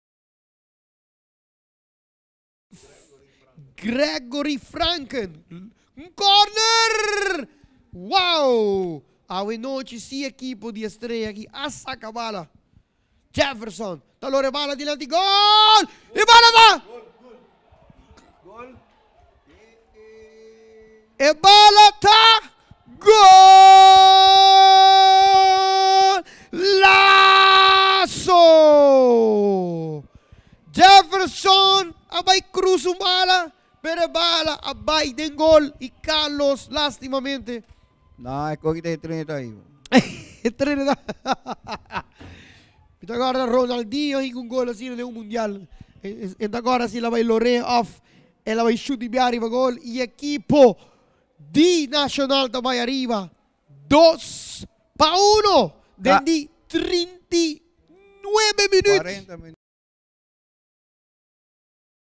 Jefferson Vegas' goal would win it for Nacional / Washington
Jefferson Vegas would try and cross a ball and would find the net after a mistake of goalkeeper Carlos Geerman